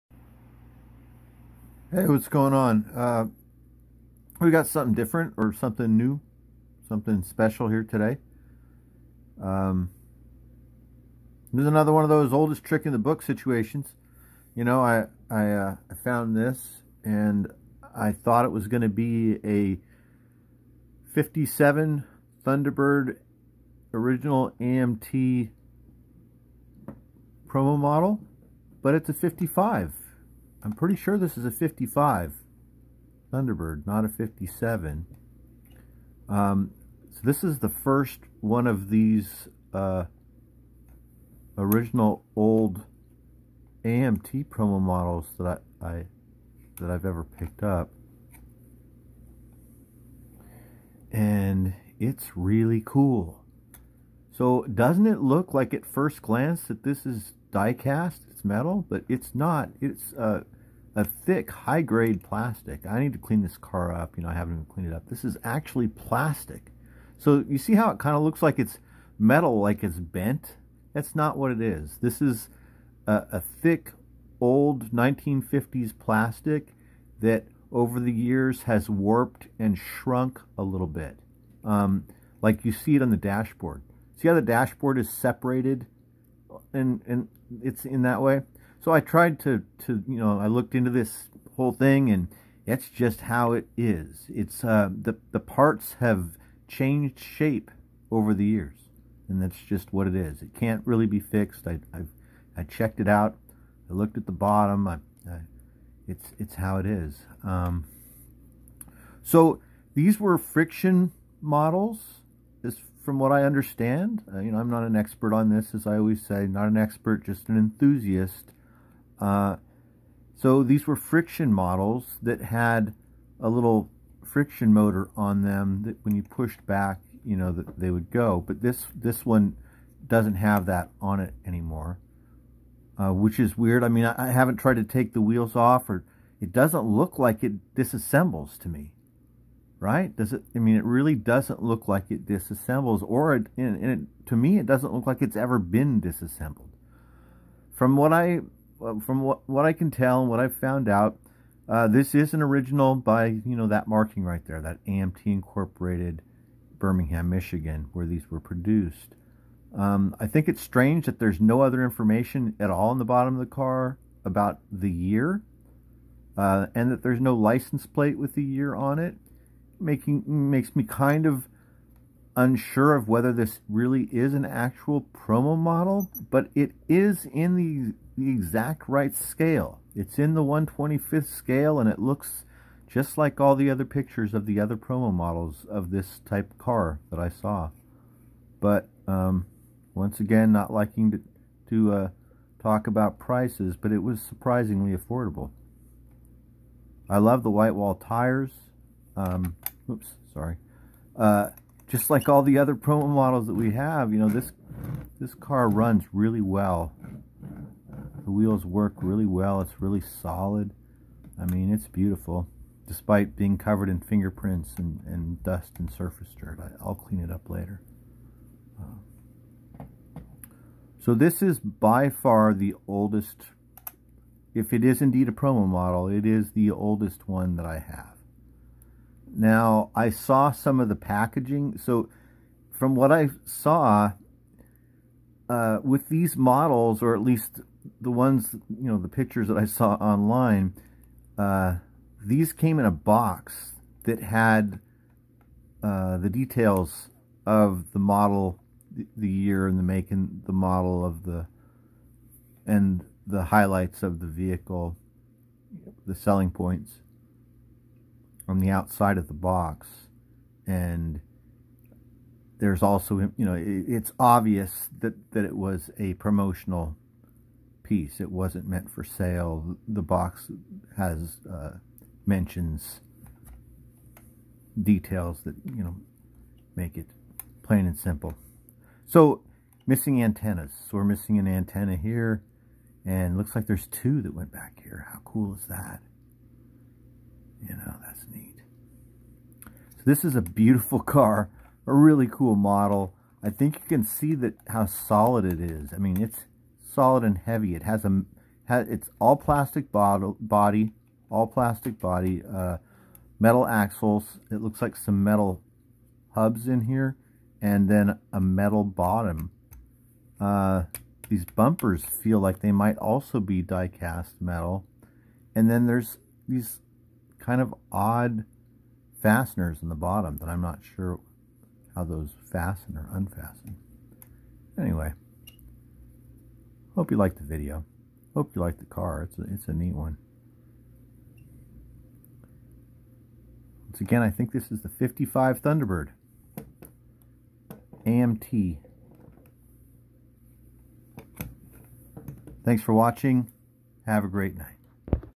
AMT '55 Ford Thunderbird Vintage Promo Model
We take a look at and talk about a '55 Thunderbird  promotional car model made by AMT of Birmingham, Michigan.